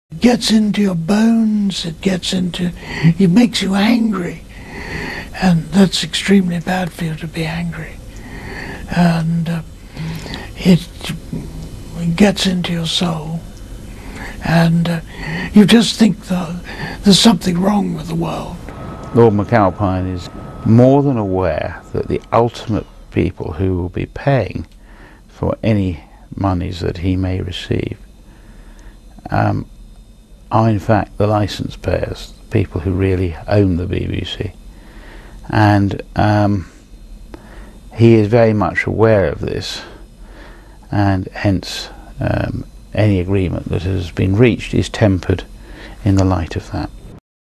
Lord McAlpines revenge Twitter slurs BBC pays 185k damages Newsnight report
Lord McAlpine is taking landmark legal action against internet gossips who falsely branded him a paedophile.
Lawyers for the Tory peer warned Twitter users 'we know who you are' and urged them to come forward voluntarily or face being pursued through the courts.
His action is intended to stop so-called 'trial by Twitter' and, if successful, could radically change the way the internet is policed and make those using social networks more directly accountable for defamatory comments.
Lord McAlpine, the former Tory party treasurer wrongly accused of being a child abuser following a botched Newsnight report, yesterday agreed a £185,000 compensation settlement with the BBC -- funded by licence-payers.